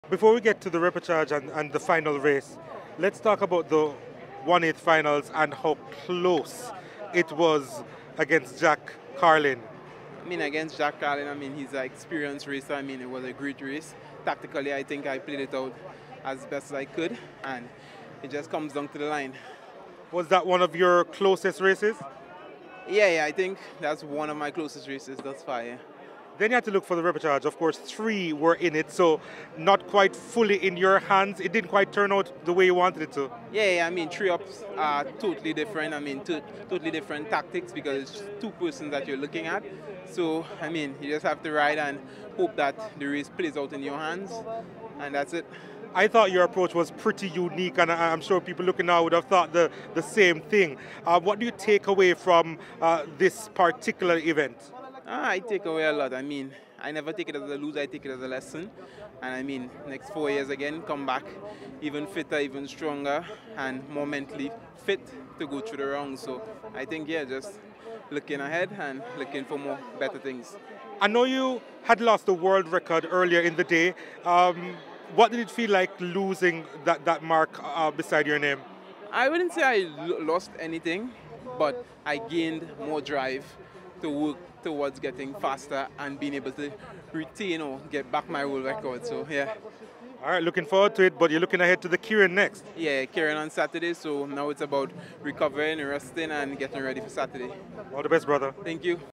Paris 2024 | T&T's Paul: "In the next 4 years I'll come back even fitter and stronger" | SportsMax
Trinidad and Tobago's Nicholas Paul: "In the next 4 years I'll come back even fitter and stronger"